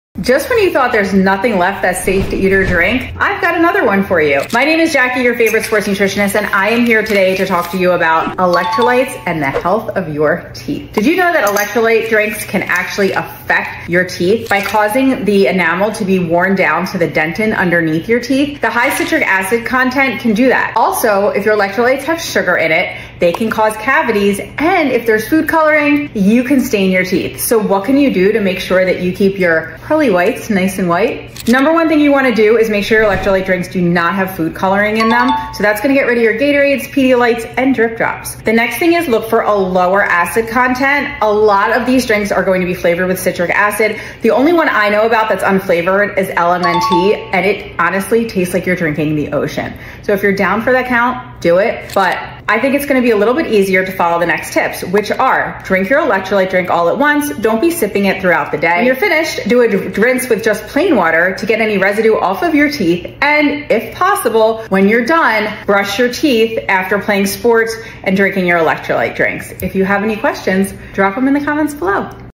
Are Electrolytes Bad for Your Teeth?
Electrolytes may be harming your teeth. But have no fear I have some tips on how to prevent that!

🍋 The citric acid can affect the enamel on your teeth .

🍰 Added sugar can cause cavities

🦷 Food coloring contain your teeth

If you aren’t an athlete - look for a product that is low and sugar or sugar-free, if you are performance athlete, you do need that sugar to fuel your workouts .

Definitely avoid anything with food coloring. Dyes are bad for you anyway.

And try to drink your electrolyte drink pretty quickly don’t sip on it all day . And if you can run your mouth with plain water after drinking electrolytes.